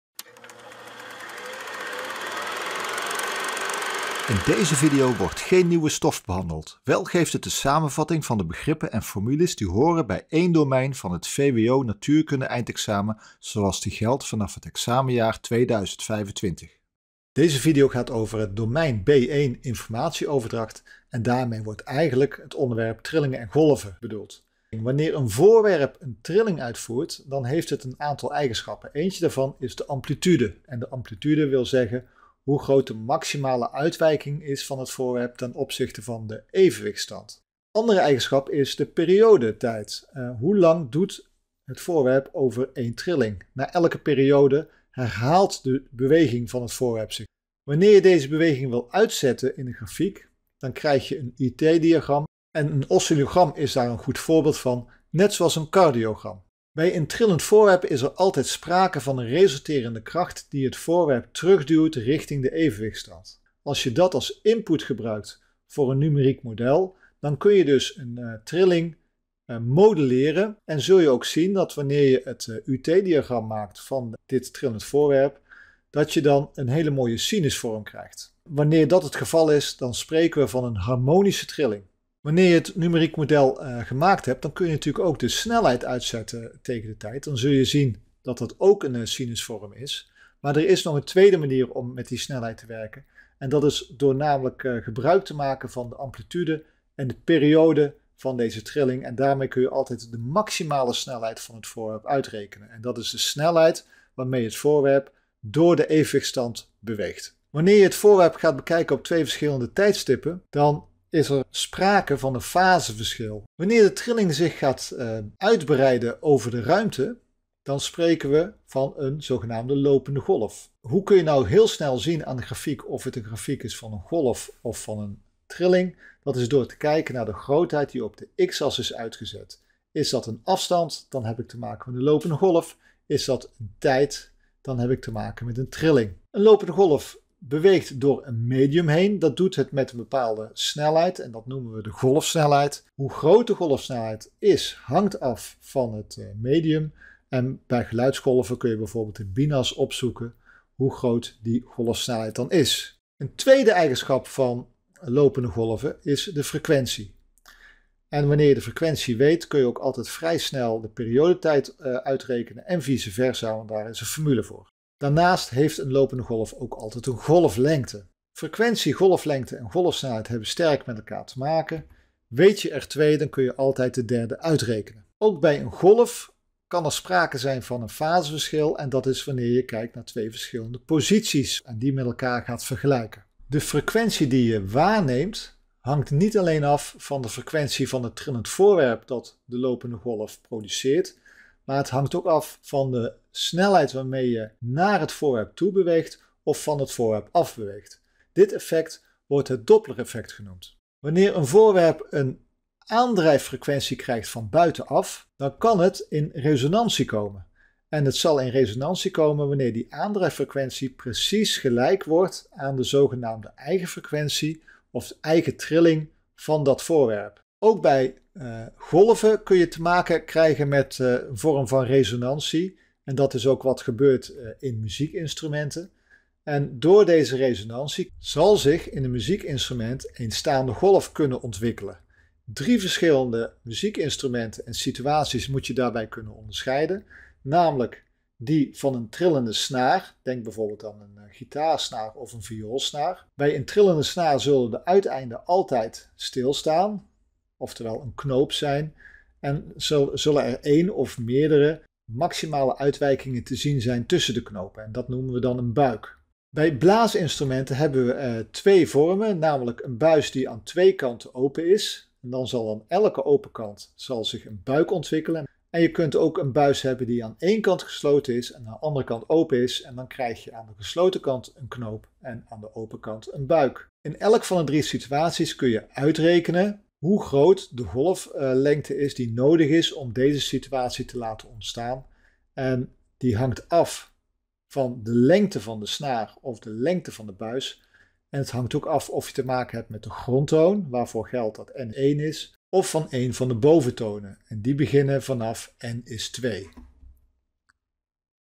Samenvatting 2025 B1 informatieoverdracht
In deze samenvatting worden alle belangrijke termen en formules uit het domein B1 behandeld, geldend voor het VWO examenprogramma vanaf 2025.

Gebruik de concept map (PDF) bij het leren van de termen en hun onderlinge samenhang. De concept map (Powerpoint) kan ook door de docent ingezet worden als advance organizer.